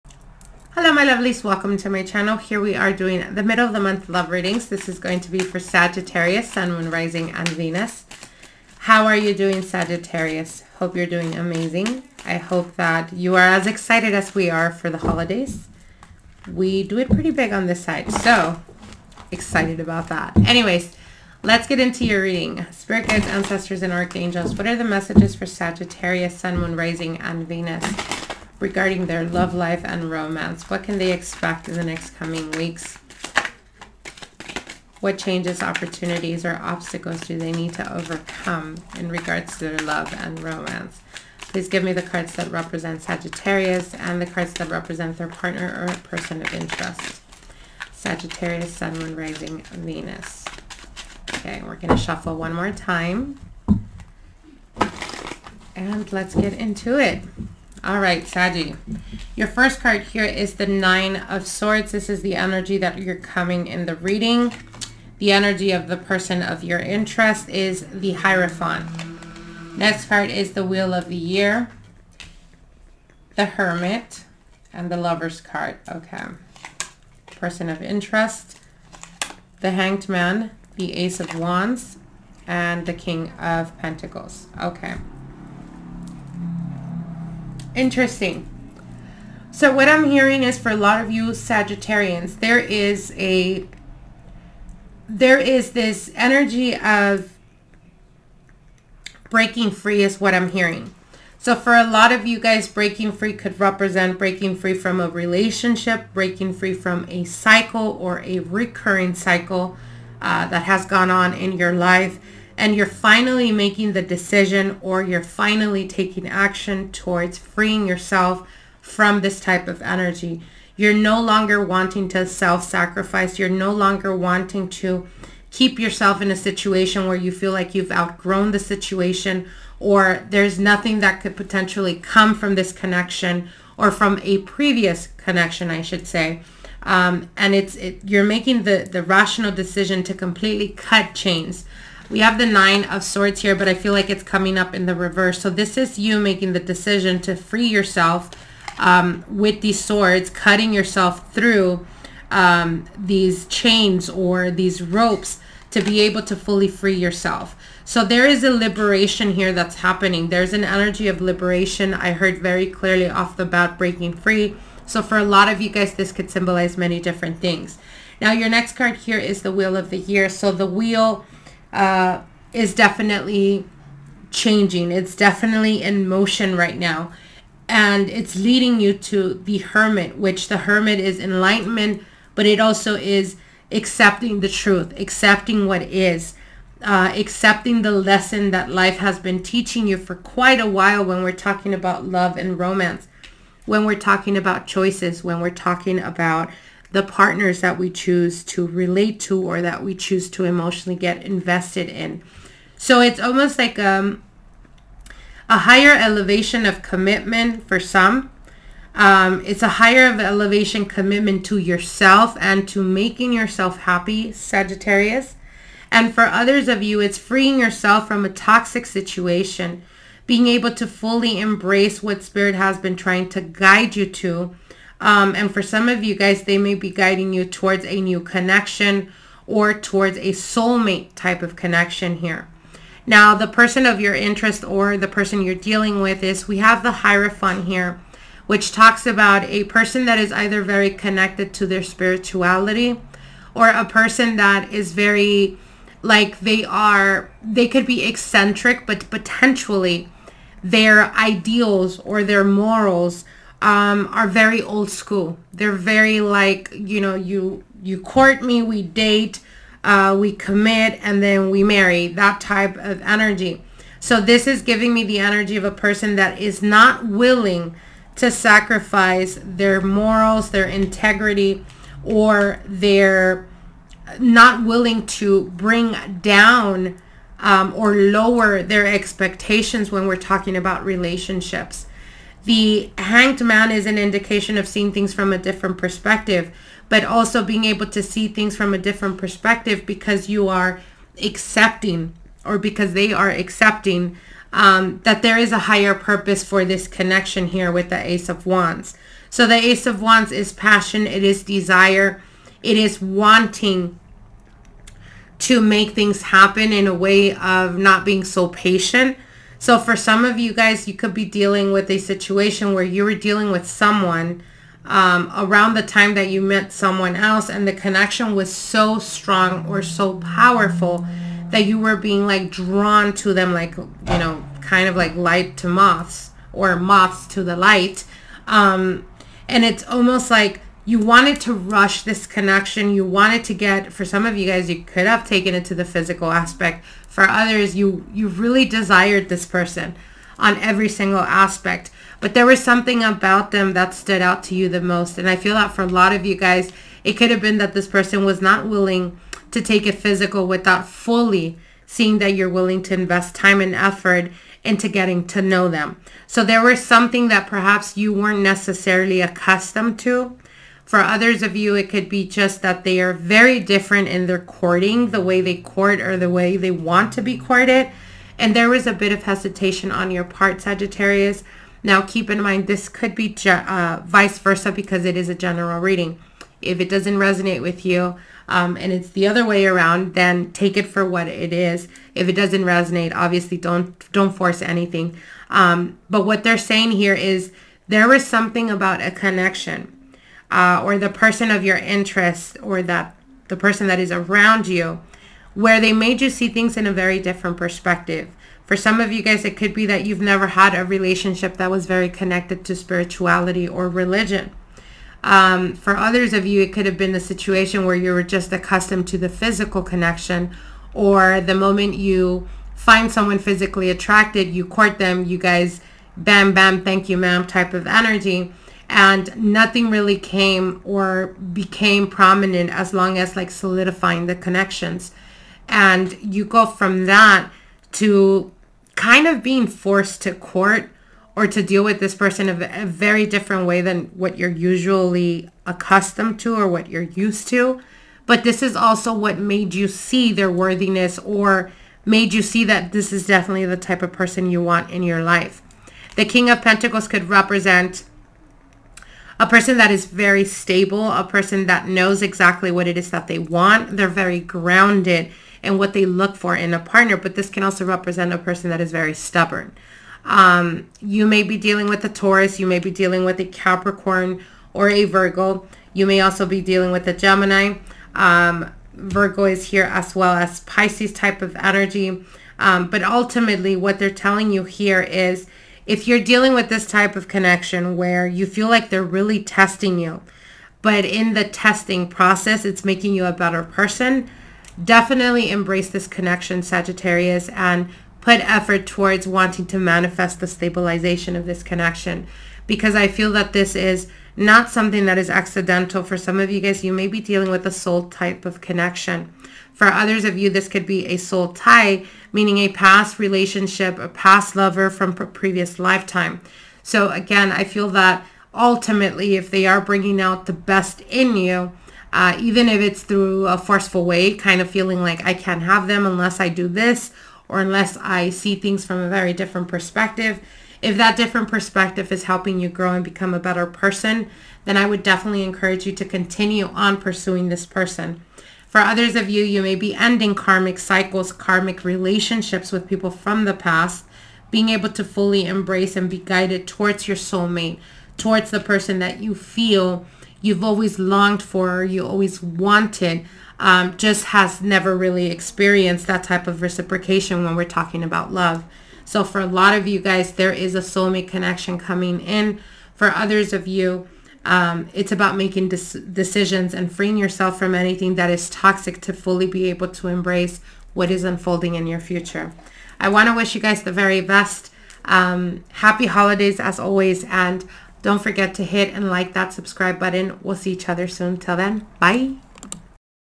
SAGITTARIUS- NOVEMBER LOVE 2021 "SOULMATE COMING IN"
Hello my lovelies here are the middle of the month tarot love readings for all zodiac signs for the month of November 2021 lets see whats unfolding and what spirit has to say about your sign!

3 Card Reading
30 Min Reading
1 Hr Reading